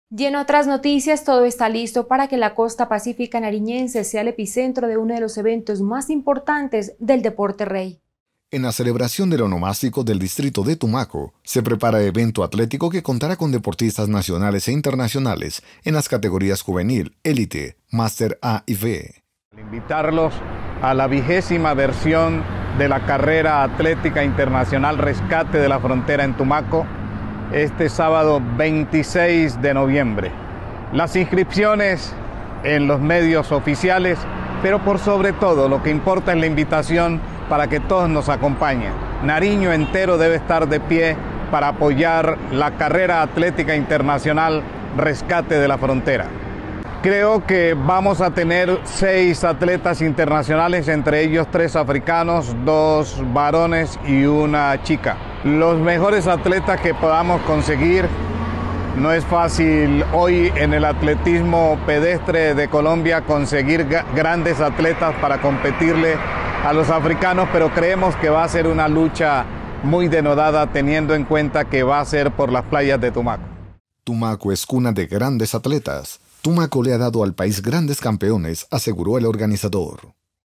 🔴ACTUALIDAD INFORMATIVA| XX Carrera Atlética Internacional “Rescate de la frontera” en Tumaco.
🔴ACTUALIDAD INFORMATIVA| Este 26 de noviembre se realizará la XX Carrera Atlética Internacional “Rescate de la frontera” en Tumaco.